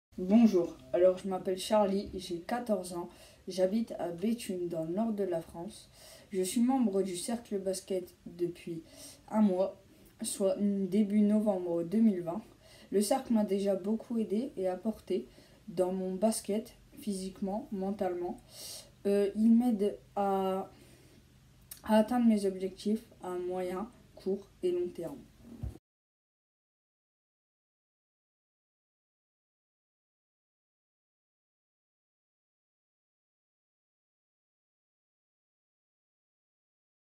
Charly témoigne sur le Cercle Basket
Apprendre, être suivi et bien plus encore avec le Cercle Basket. Nous recherchons des joueurs.